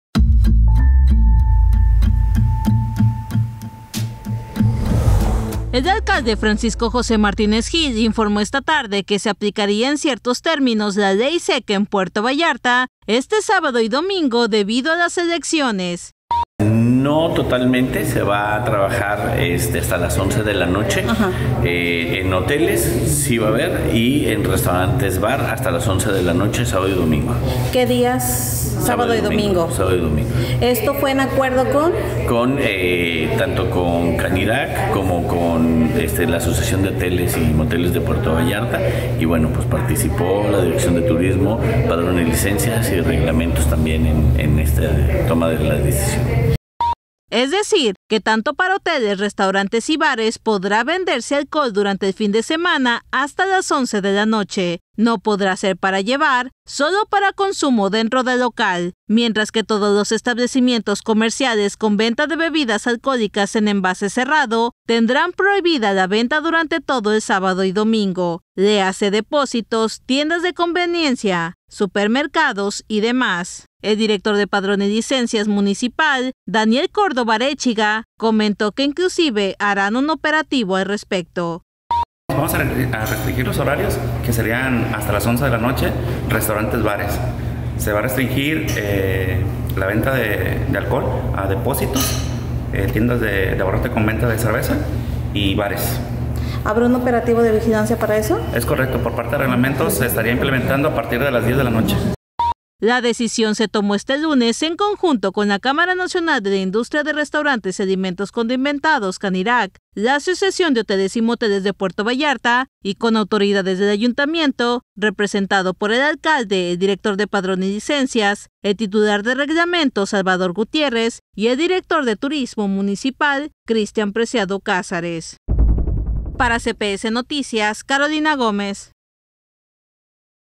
Habrá Ley Seca a medias en Puerto Vallarta por las elecciones 2024 | CPS Noticias Puerto Vallarta
¡Sin alcohol en las elecciones2024! 🍻❌ Este sábado y domingo habrá ley seca en la ciudad, así lo confirmó el alcalde interino. 👇